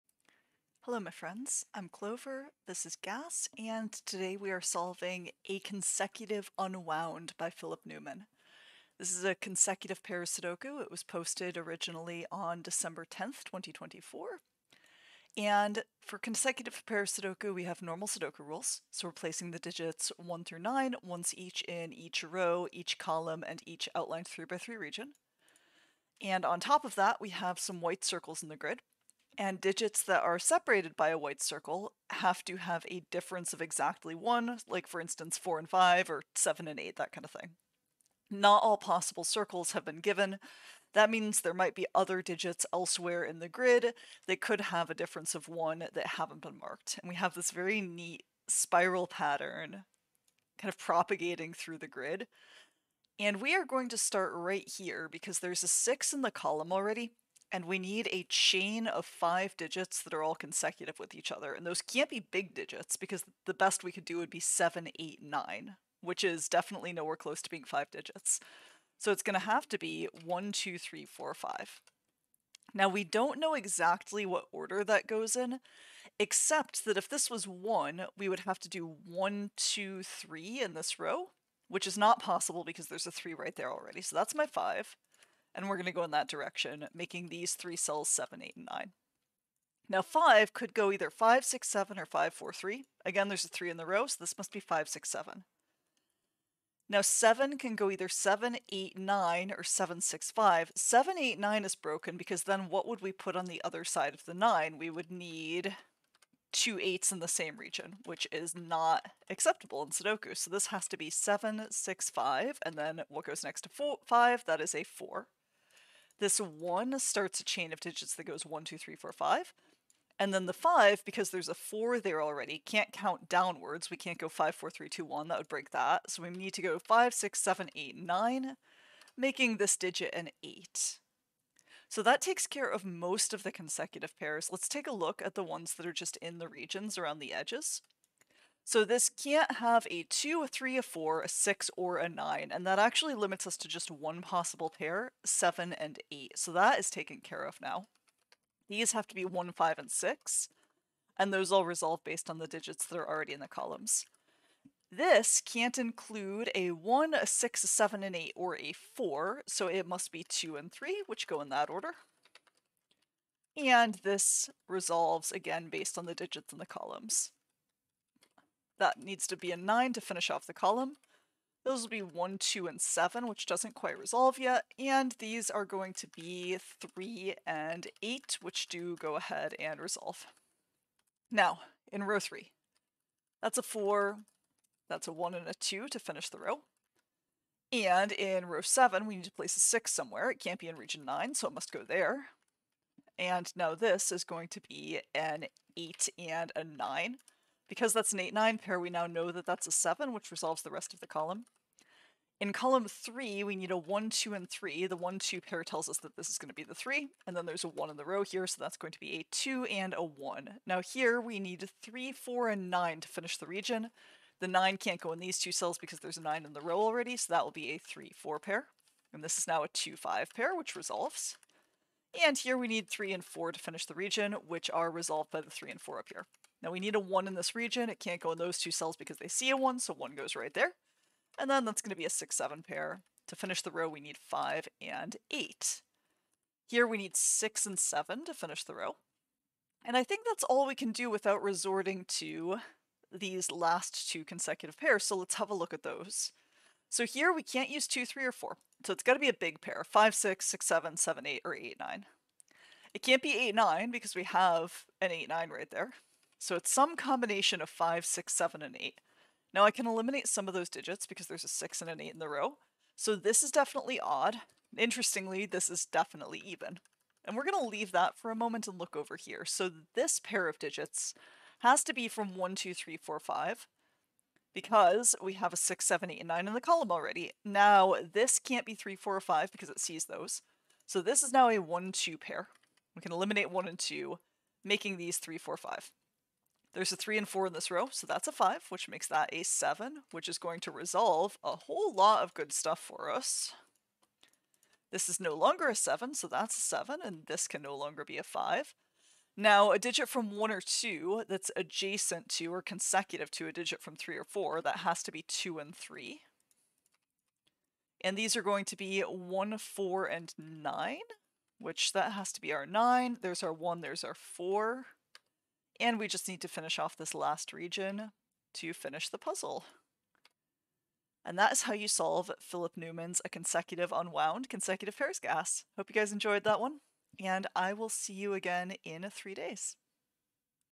GAS Sudoku Walkthrough - A Consecutive Unwound by Philip Newman (2024-12-10)
Clover solves Philip's sudoku "A Consecutive Unwound", originally posted on December 10, 2024.

RULES:
Consecutive Pairs: Digits in cells separated by a white dot must be consecutive (have a difference of 1). (No negative constraint - other pairs of digits may also be consecutive.)

HAT TIMES:
6:30/12:30